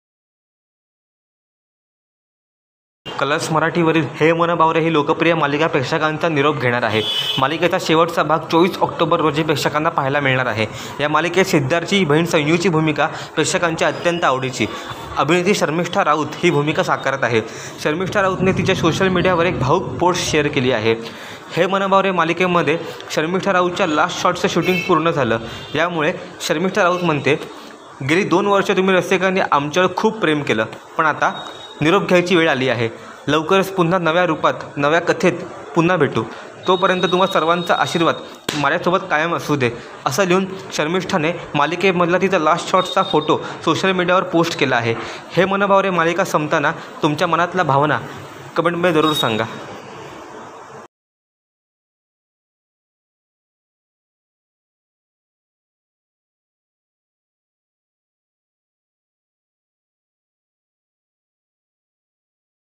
मालिकेचा शेवटचा शॉट देताना संयू झाली भावूक | He Mann Baware, Sharmistha Raut
कलर्स मराठी वरील हे मन बावरे ही लोकप्रिय मालिका प्रेक्षकांचा निरोप घेणार आहे. मालिकेचा शेवटचा भाग 24 ऑक्टोबर रोजी प्रेक्षकांना पाहायला मिळणार आहे. या मालिकेत सिद्धार्थ ची बहीण संयु ची भूमिका प्रेक्षकांच्या अत्यंत आवडीची अभिनेत्री शर्मिष्ठा राऊत ही भूमिका साकारत आहे. शर्मिष्ठा राऊत नाही तिच्या सोशल मीडियावर एक भावूक पोस्ट केली आहे.